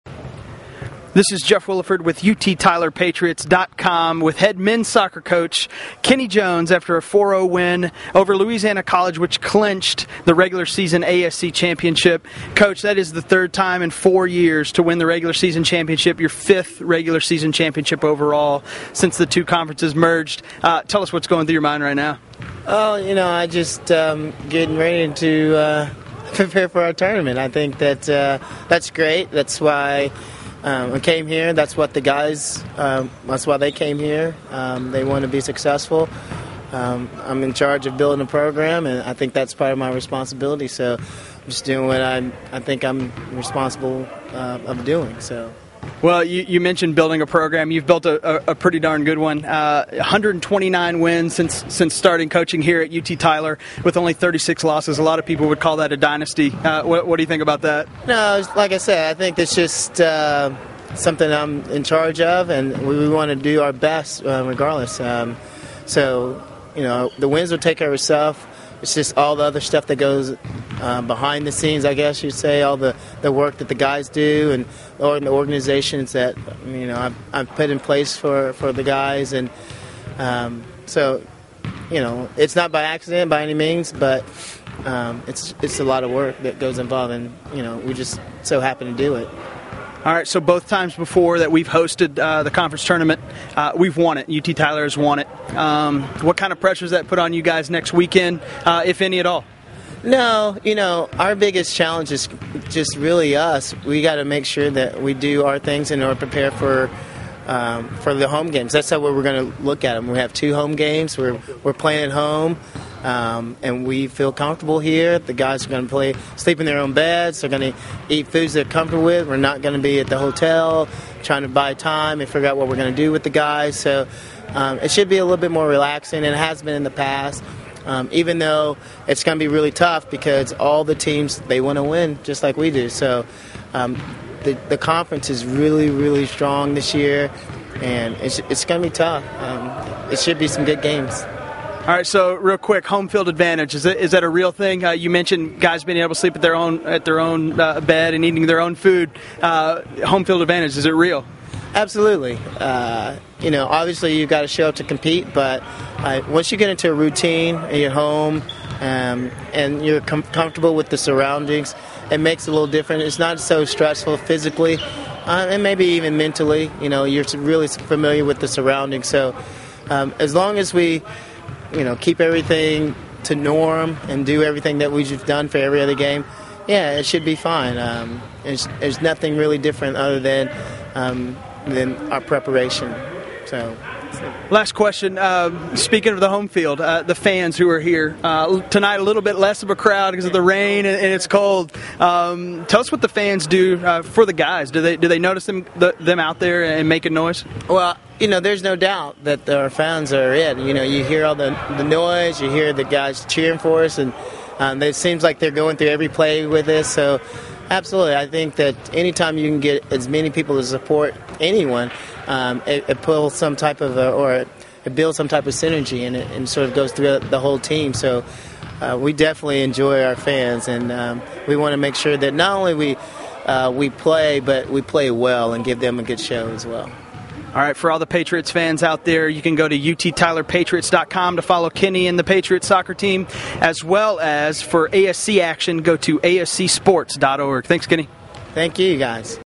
Kenny Jones - UT Tyler Men's Soccer (Oct. 27, 2011)
TYLER -- The UT Tyler men's soccer team won their fifth American Southwest Conference regular season championship with a 4-0 shutout of Louisiana College Thursday on Senior Night at the Citizens 1st Bank-Perkins Soccer Complex.  Fittingly, all four of the Patriots' goals were scored by seniors.

The title, which is the Patriots' second consecutive and third in the last four years, earns them the right to host the ASC Championship Tournament set for Nov. 4-6.  The winner of the tournament earns the league's automatic qualifier to the NCAA Championships.

UT Tyler (14-2, 10-1 ASC), ranked third in the NCAA West Region and No. 12 in the NSCAA national poll, is the defending ASC champion.

The Patriots first goal came on the foot of Victor Almendariz in the 24th-minute. Luis Jaimes recorded the assist on the goal and UT Tyler held the 1-0 lead into halftime.

After the break, Almendariz scored again, this time in the 60th minute assisted by Alberto Munoz. The Patriots extended their lead to 3-0 on a free kick by Jake Urbaniak at 69:55.

Senior Jonatan Benitez scored his first goal of the season in the 83rd minute to give the Patriots their final margin of 4-0. He was assisted by Elijah Shipman.

UT Tyler expected to be playing for the title on Saturday in a showdown with Mississippi College, but the Choctaws tied with Concordia on Thursday which dropped them out of contention in the tight ASC standings.

The Patriots finished the night with a 26-4 shot advantage. UT Tyler keeper Sergio Gardea made four saves.

The tournament bracket will be set following the completion of the regular season on Saturday.